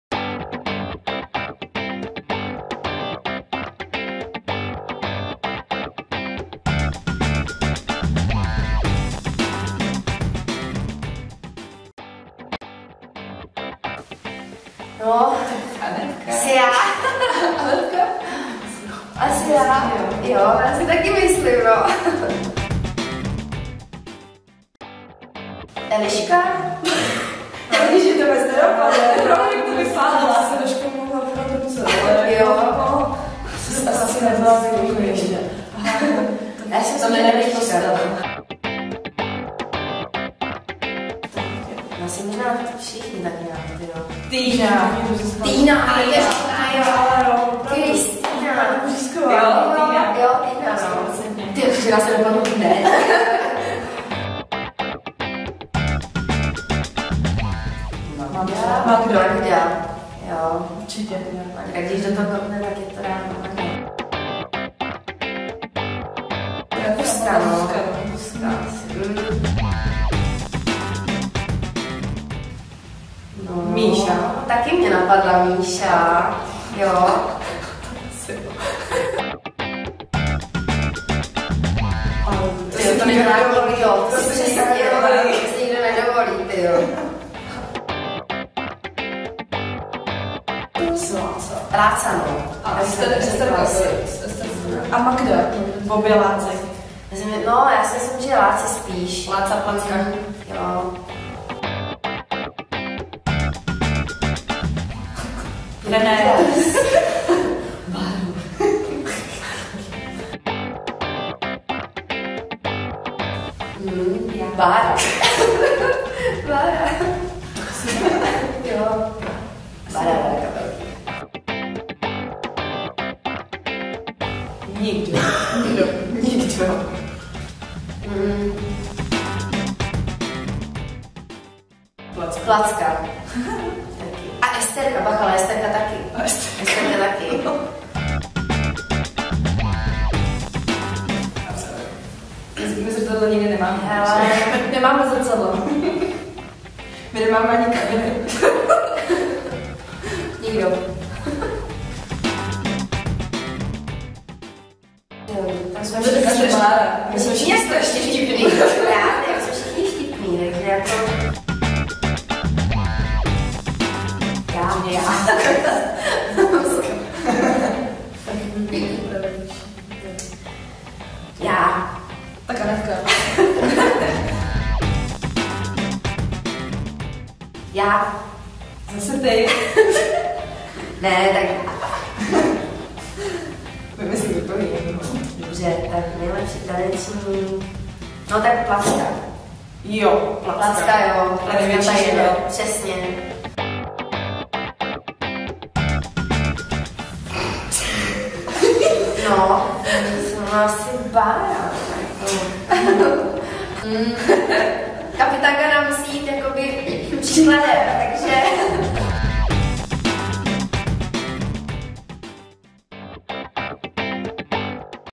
3v1 LTRZ - Bojovník Helga s Anet a rumovou královnou Miky.
3v1 LTRZ - Jak odpovídaly Zahořanské Ladies na otázky ohledně "NEJ" v jejich týmu? V sestřihu uvidíte Báru Neprašovovou, Míšu Blažovou a Anet Kratochvílovou.